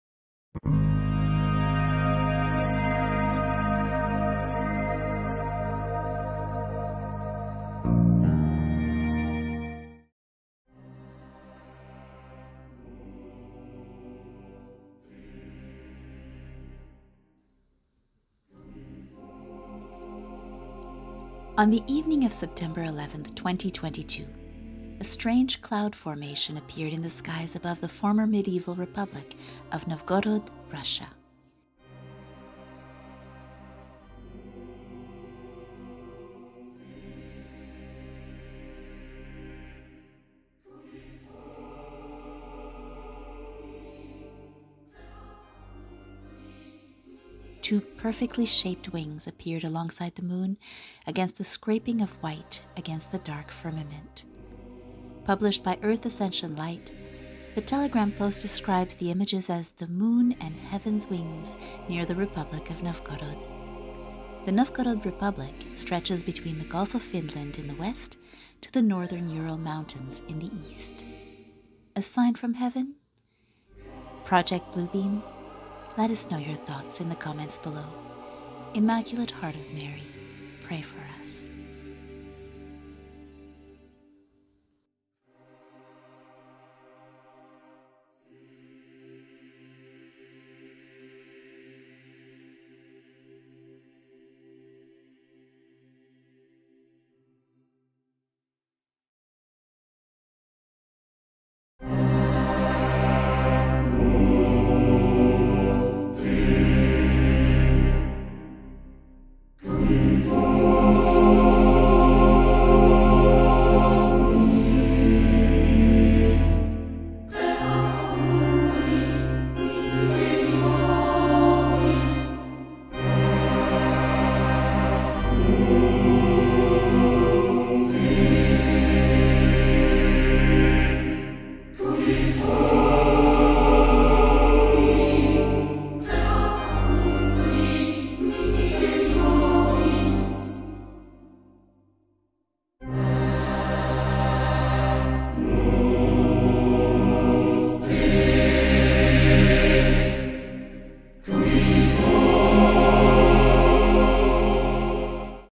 Two Perfectly-Shaped Wings Appear in Novgorod, Russia! Amazing Photos! Eye-Witness: Heaven’s Wings!!
On the evening of September 11, 2022, a strange cloud formation appeared in the skies above the former medieval Republic of Novgorod,  Russia.

Two perfectly-shaped wings appeared alongside the moon against a scraping of white against the dark firmament.

Published by Earth Ascension Light, the Telegram post describes the images as "The moon and Heaven's wings near the Republic of Novgorod."

The Novgorod Republic stretches between the Gulf of Finland in the west to the northern Ural Mountains in the east.

Immaculate Heart of Mary, pray for us.